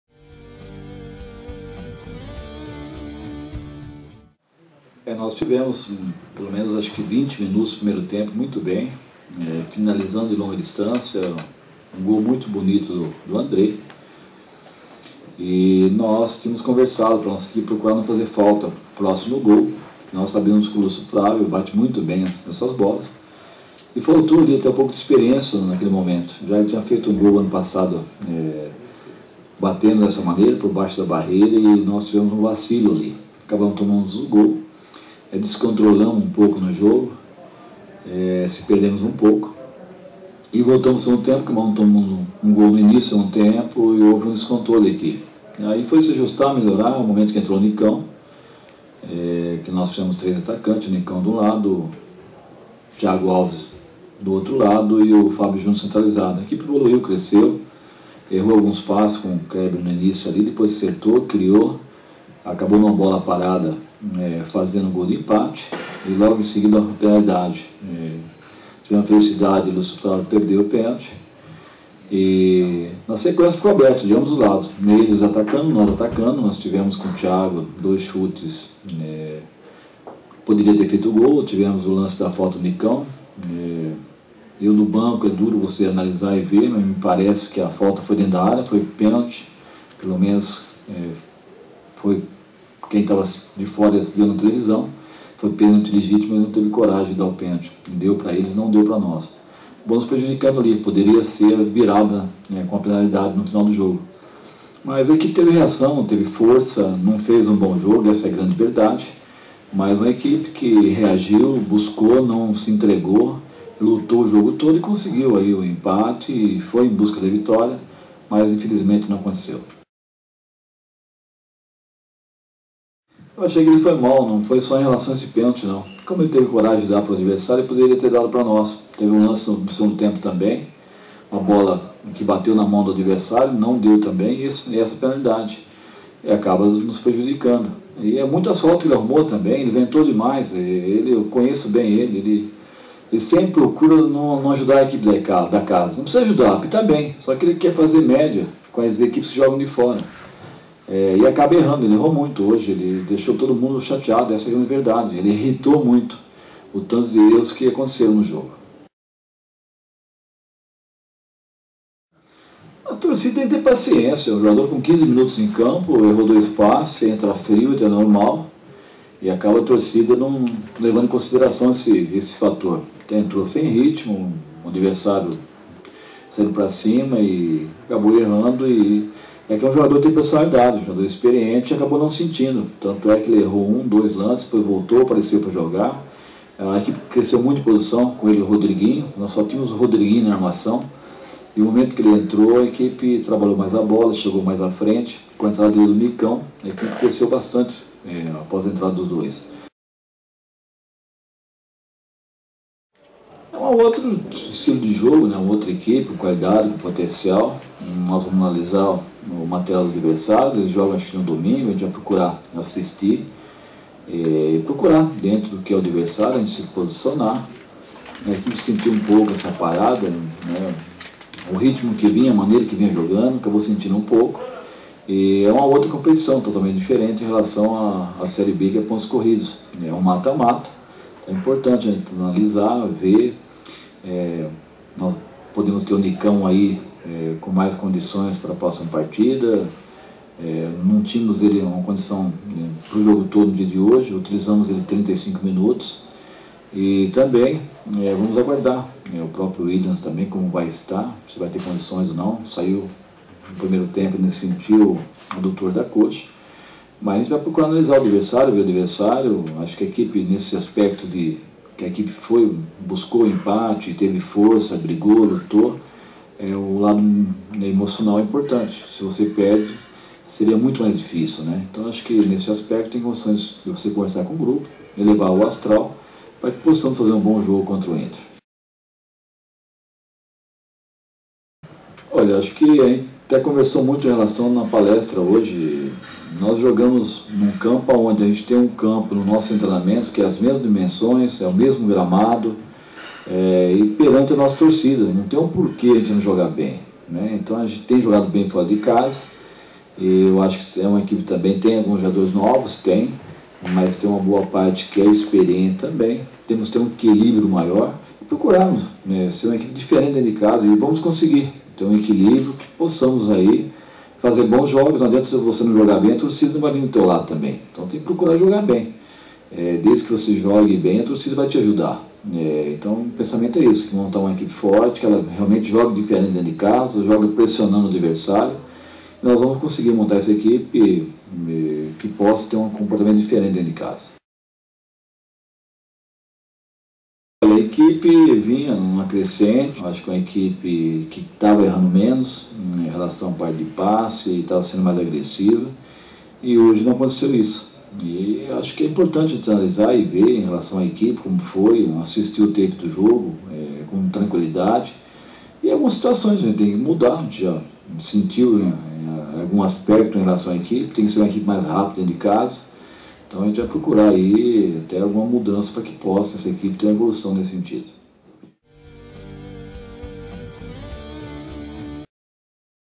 ENTREVISTA PÓS-JOGO - América 2 x 2 Paraná
Tecnico americano comenta partida válida pelo Campeonato Brasileiro Série B.